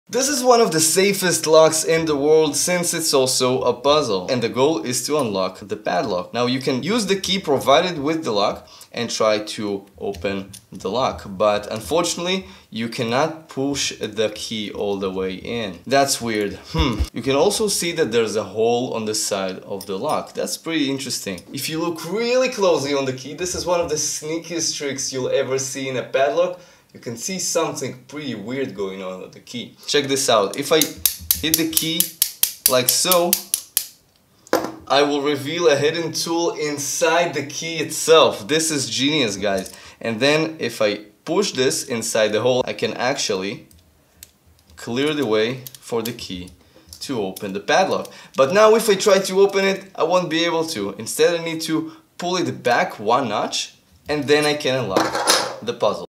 This Lock Puzzle is so SNEAKY! - B-Lock
The B-Lock by Boaz Feldman from Puzzlocks. It's so evil!